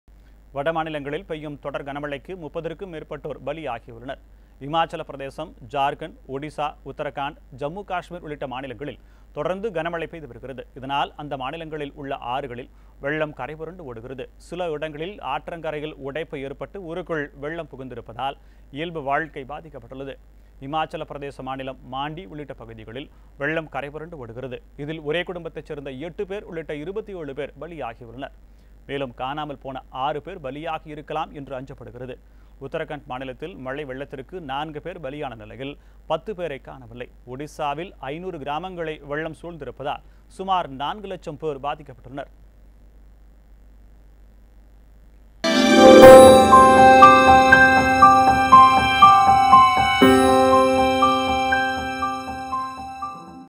வட மாநிலங்களில் கனமழையால் ஆறுகளில் வெள்ளப் பெருக்கு | NewsJ
இமாச்சல பிரதேசம், ஜார்க்கண்ட், ஒடிசா, உத்தரகண்ட், ஜம்மு காஸ்மீர் உள்ளிட்ட மாநிலங்களில் தொடர்ந்து கனமழை பெய்து வருகிறது. இதனால் அந்த மாநிலங்களில் உள்ள ஆறுகளில் வெள்ளம் கரைபுரண்டு ஓடுகிறது. சில இடங்களில் ஆற்றங்கரையில் உடைப்பு ஏற்பட்டு ஊருக்குள் வெள்ளம் புகுந்திருப்பதால், இயல்பு வாழ்க்கை பாதிக்கப்பட்டுள்ளது. இமாச்சல பிரதேச மாநிலம் மாண்டி உள்ளிட்ட பகுதிகளில் வெள்ளம் கரைபுரண்டு ஓடுகிறது. இதில் ஒரே குடும்பத்தை சேர்ந்த 8 பேர் உள்ளிட்ட 27 பேர் பலியாகியுள்ளனர். மேலும் காணாமல் போன 6 பேர் பலியாகியிருக்கலாம் என அஞ்சப்படுகிறது. உத்தரகண்ட் மாநிலத்தில் மழை வெள்ளத்துக்கு 4 பேர் பலியான நிலையில், 10 பேரை காணவில்லை. ஒடிசாவில் 500 கிராமங்களை வெள்ளம் சூழ்ந்திருப்பதால், சுமார் 4 லட்சம் பேர் பாதிக்கப்பட்டுள்ளனர்.